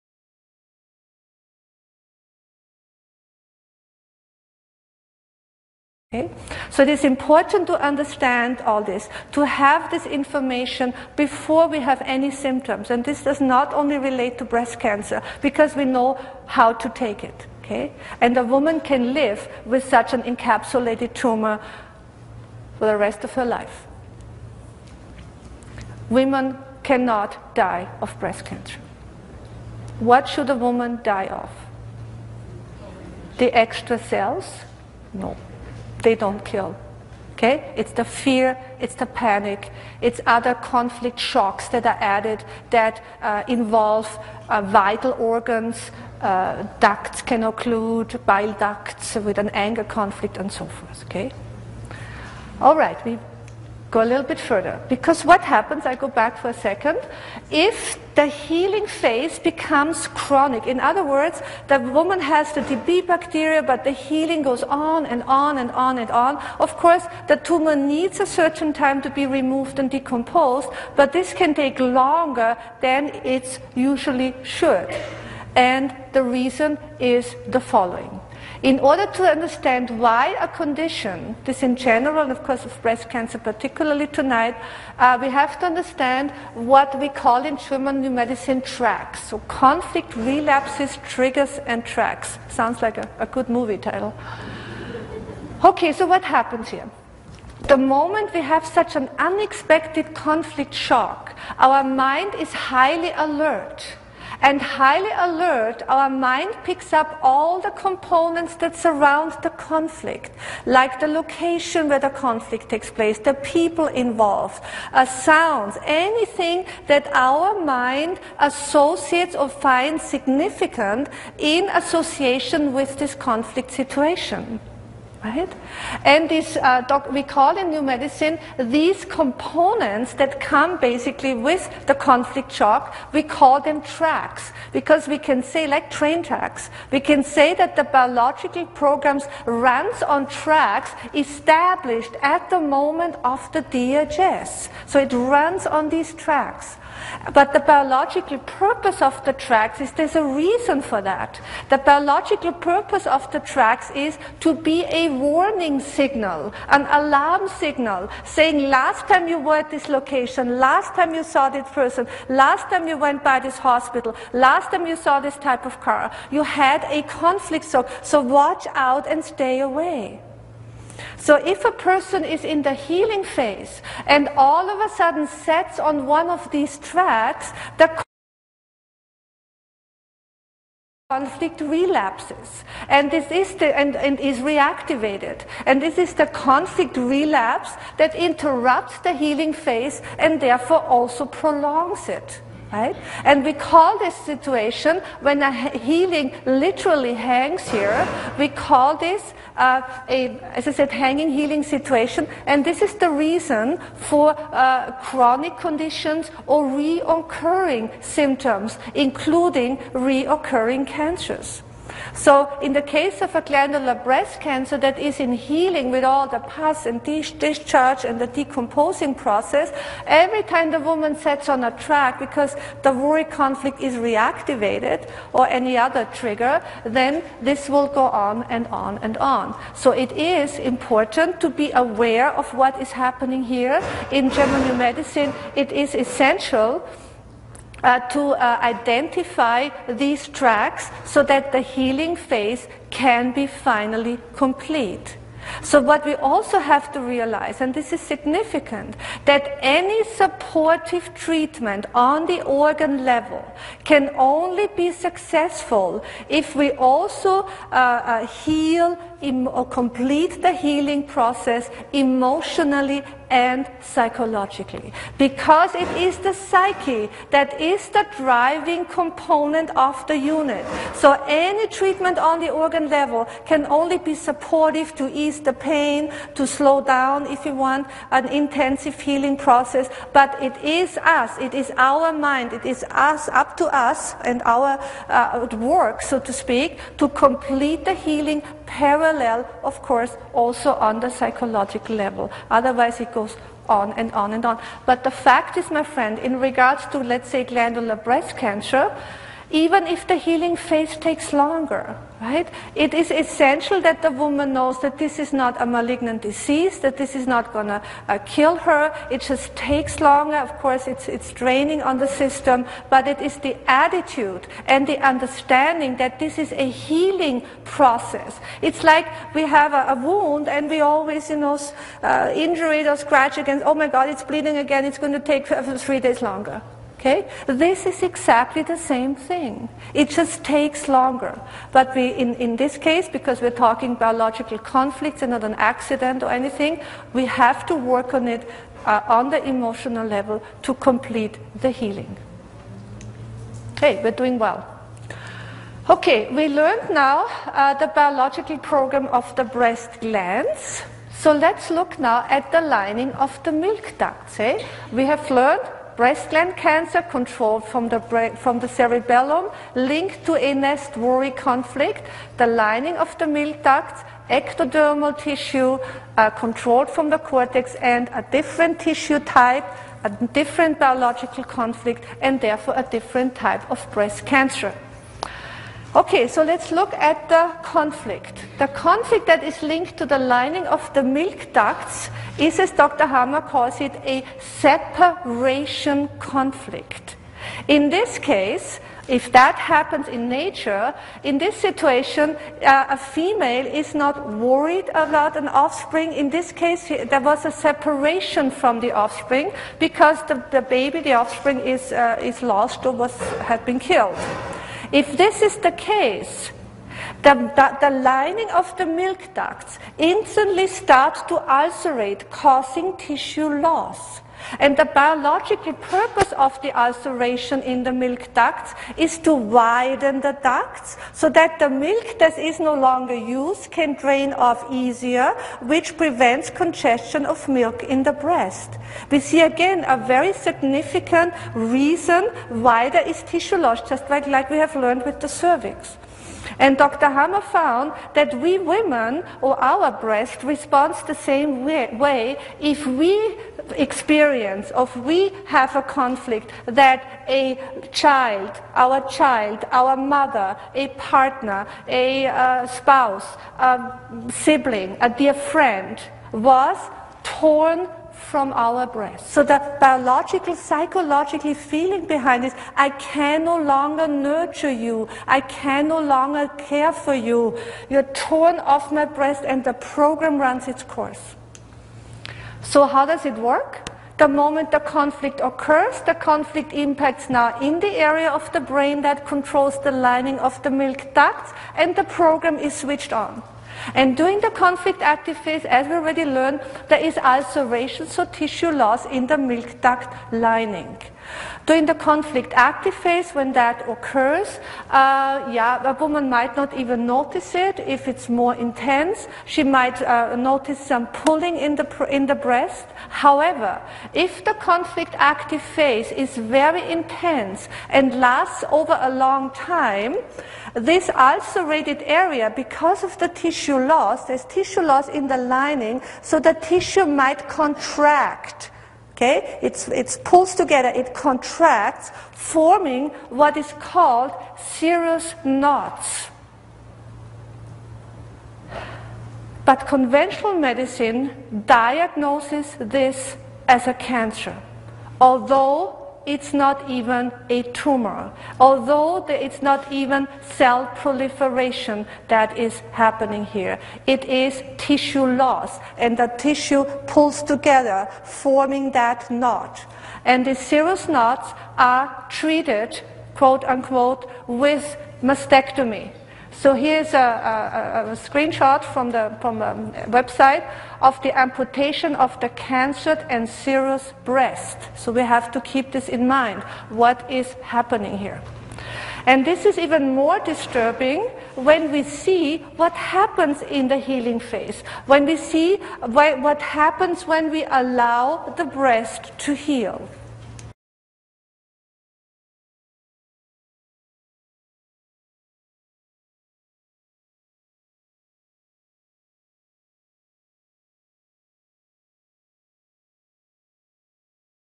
GNM - Rak piersi - cz.3/6
Wykład Caroline Markolin na temat różnych rodzajów raka piersi - wyjaśnia mechanizm ich powstawania i naturalnego zdrowienia, w oparciu o 5 Praw Natury odkrytych przez dr Geerda Ryke Hamera.